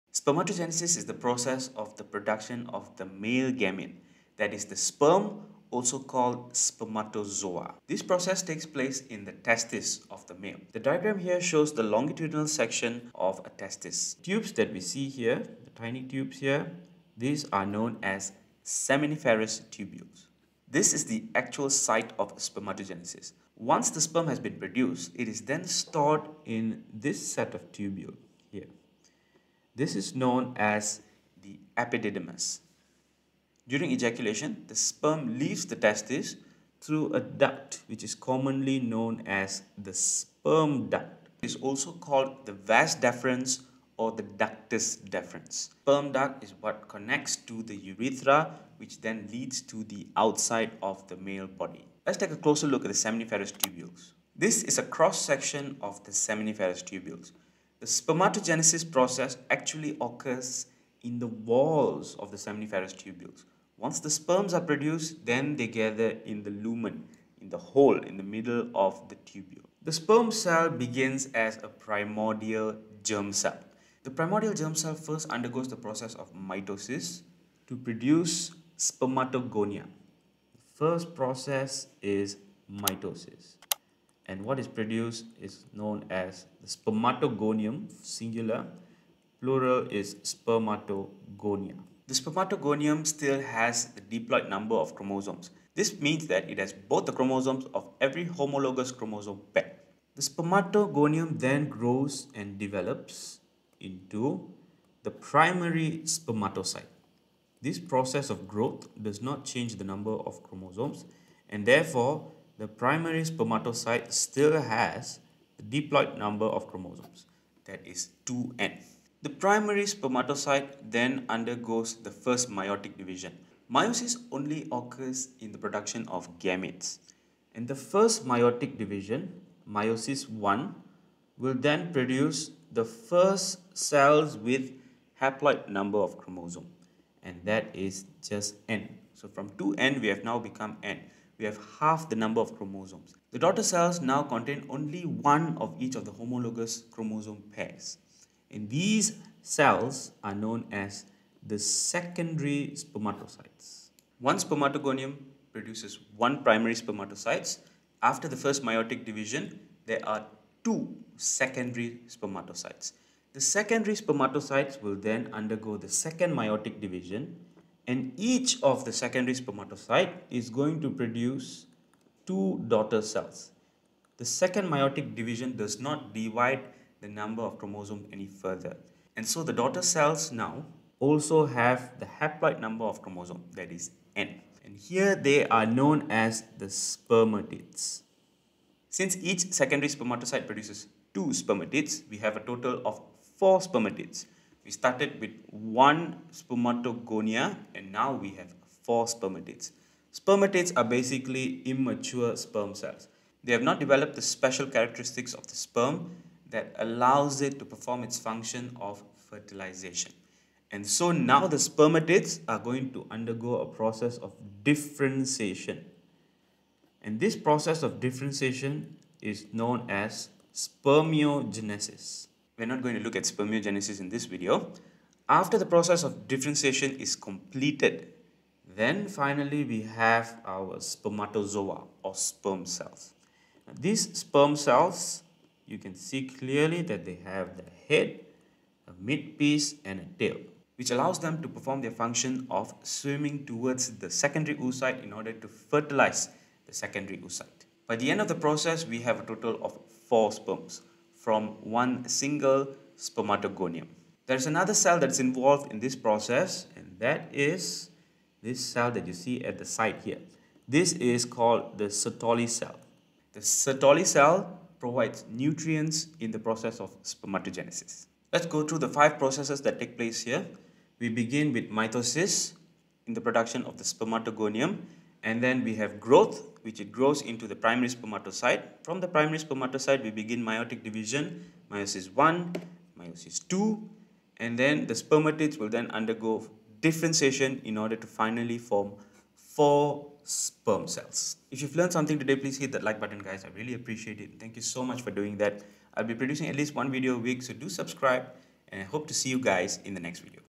Spermatogenesis
What is spermatogenesis? How are sperms produced? Where does spermatogenesis take place?

00:00 - Structure of testis
00:54 - Spermatogenesis (mitosis)
01:59 - Spermatogenesis (meiosis)
03:20 - Spermatogenesis (differentiation)
04:14 - Sertoli cells

Catering for KSSM SPM students.

This topic can be found in KSSM Biology Form 4 Chapter 15: Sexual Reproduction, Development and Growth in Humans and Animals - Gametogenesis in Humans

Notes, practice questions and more: mrrueltuition.com